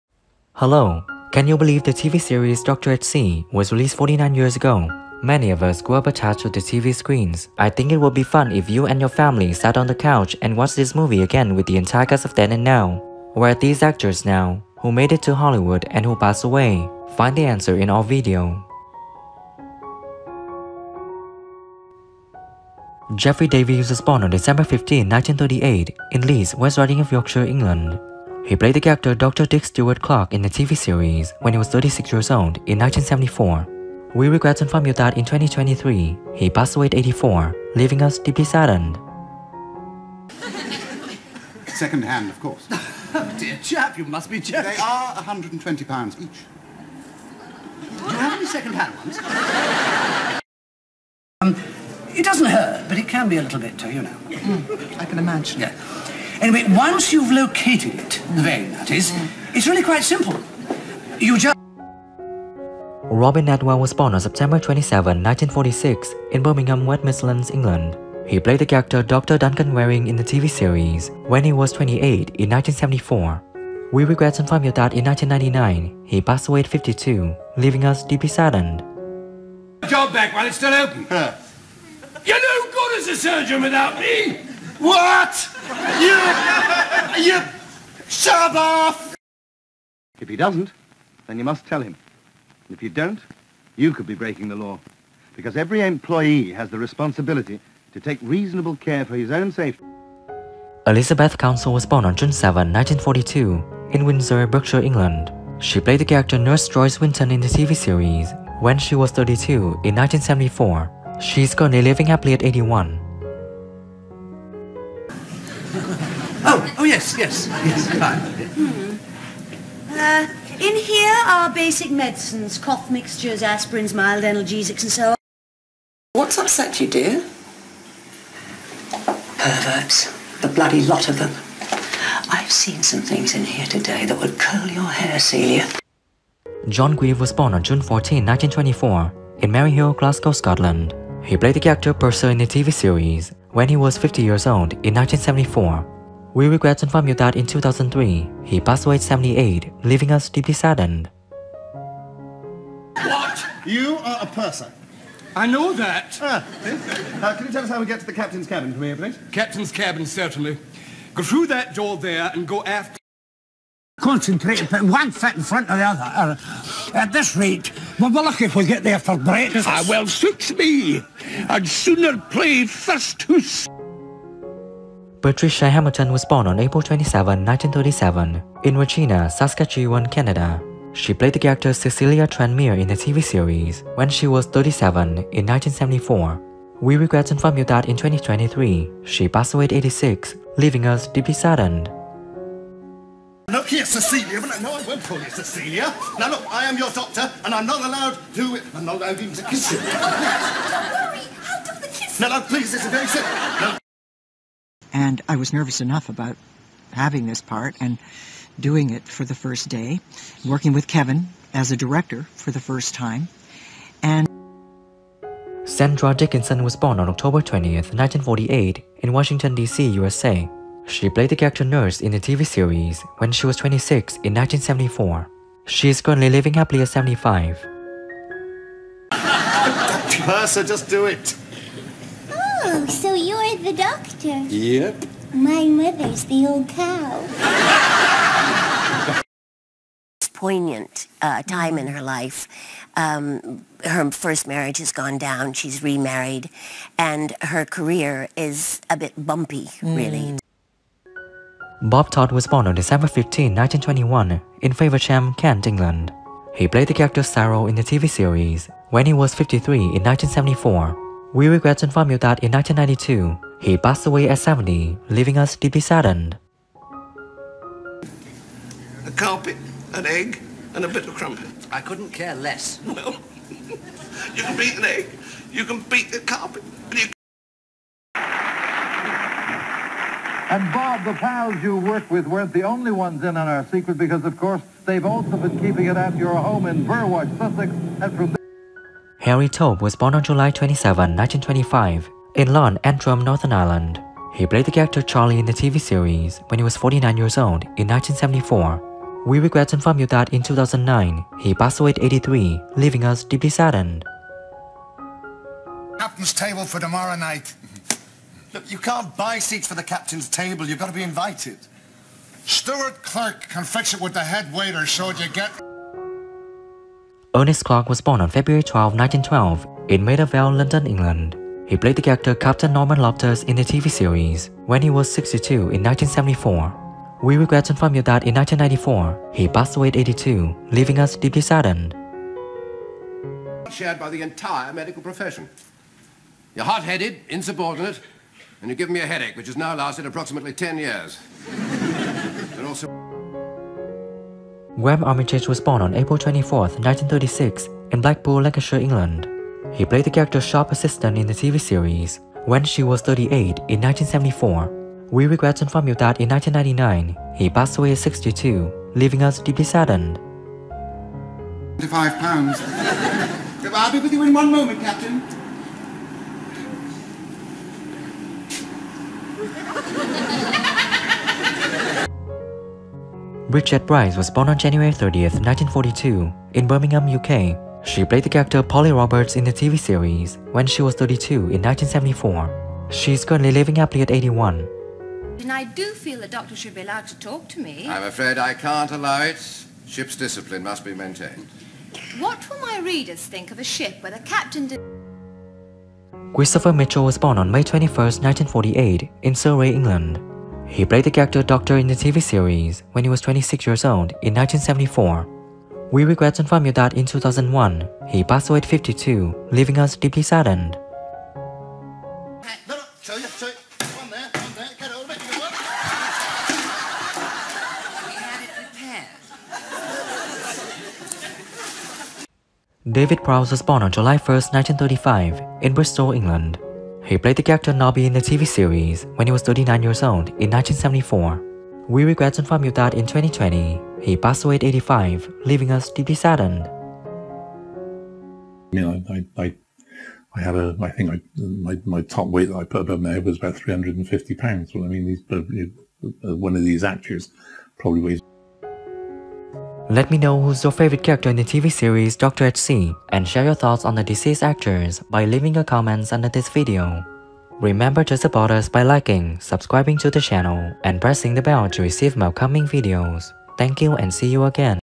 Doctor at Sea 1974 - The Whole Cast Died Tragically!
Doctor at Sea is a British television comedy series based on a set of books by Richard Gordon about the misadventures of doctors at sea.[1] The series followed directly from its predecessor Doctor in Charge, and was produced by London Weekend Television in 1974.